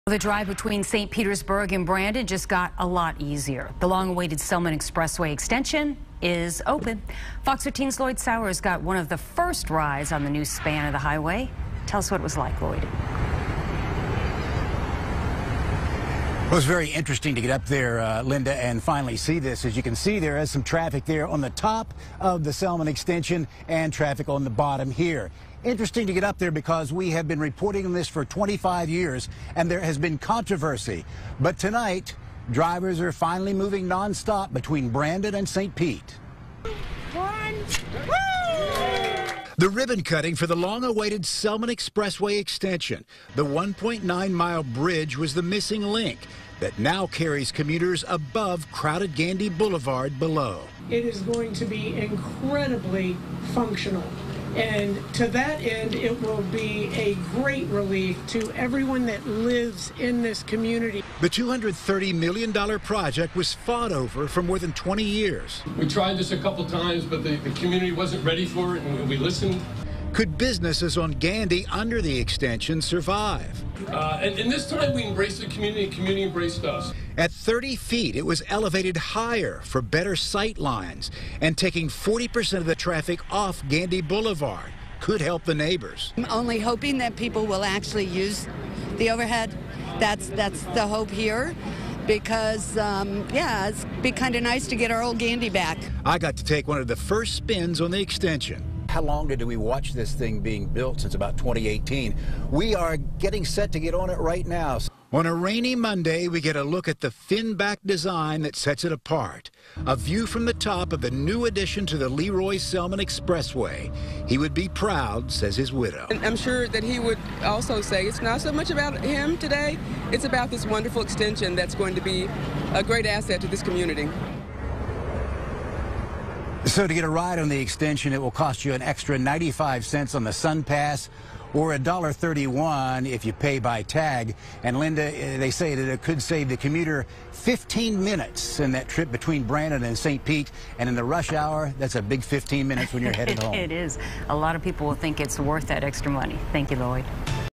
Selmon Expressway's Gandy extension opens in Tampa
The ribbon was cut Monday on the long-awaited Selmon Expressway West Extension. The 1.9-mile, $230-million bridge carries traffic above crowded Gandy Boulevard in Tampa, connecting the Lee Roy Selmon Expressway with the Gandy Bridge.

Starting in the 1990’s, there was opposition from business owners and neighbors who feared the area of Gandy Boulevard beneath the bridge would suffer economically.

To minimize possible negative impacts, the designers or the project made the elevated bridge 30 feet above the surface road instead of the standard 15 feet. Medians, planters, and the design of the concrete supports for the bridge was shown to community members.